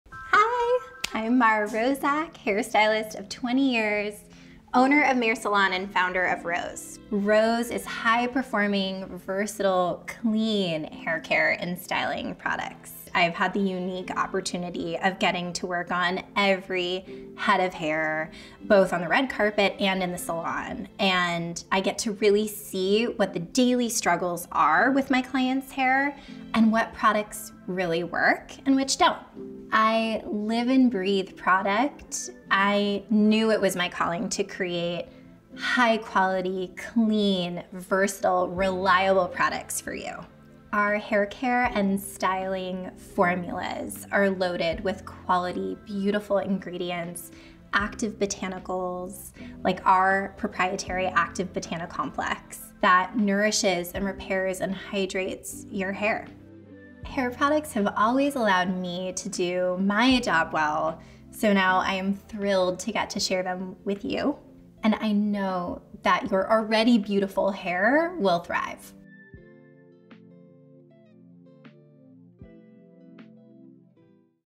Introducing RŌZ with Founder Mara Roszak
RŌZ (pronounced “rose”) is vegan, organic, sustainably-sourced and cruelty-free haircare inspired by nature and driven by performance. Meticulously crafted and mindfully-made, we strive to restore hair to its natural state: nourished and balanced. A line of curated products that make you fall in love with your hair, just as it is.